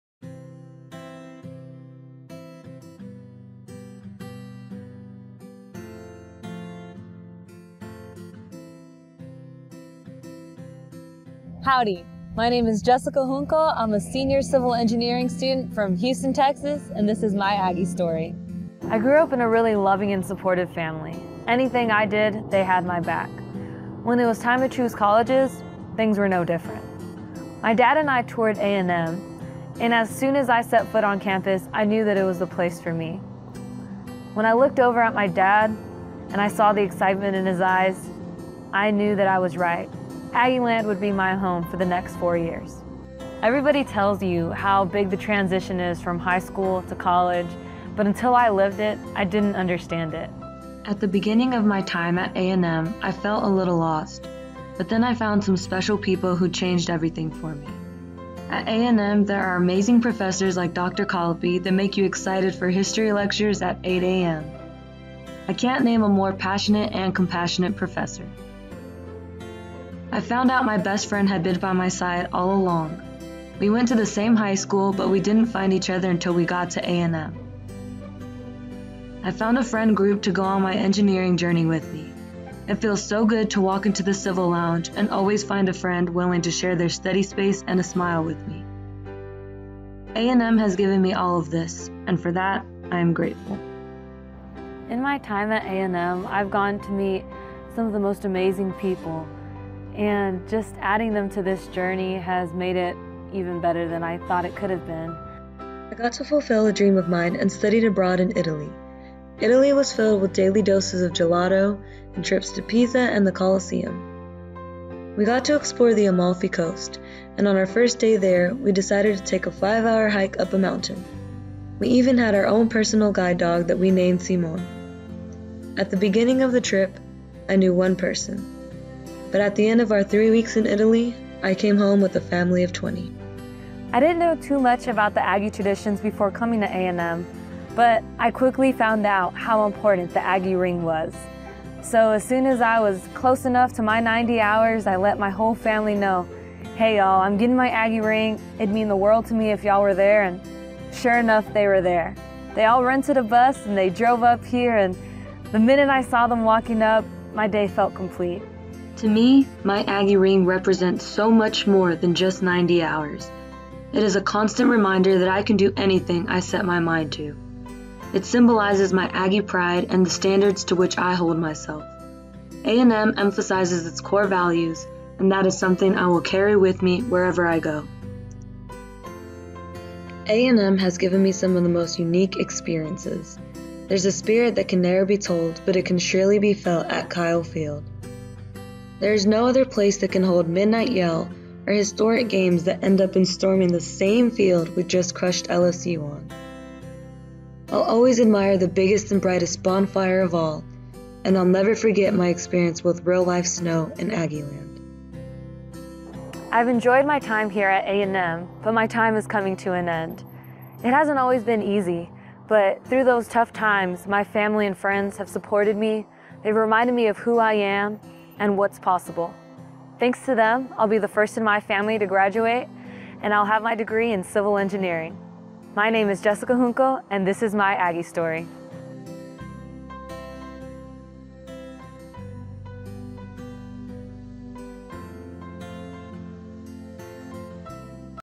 My Aggie Story: Jessica Junco shares her journey on studying engineering & studying abroad in Italy
I was born and raised in Houston, TX. I come from a loving and supportive family. When I started my time here at A&M, I wasn't sure if I could make it, especially not as an engineer. Throughout my time here though, I've gained experience, an Aggie family and have maintained the support of my family and will be graduating next May.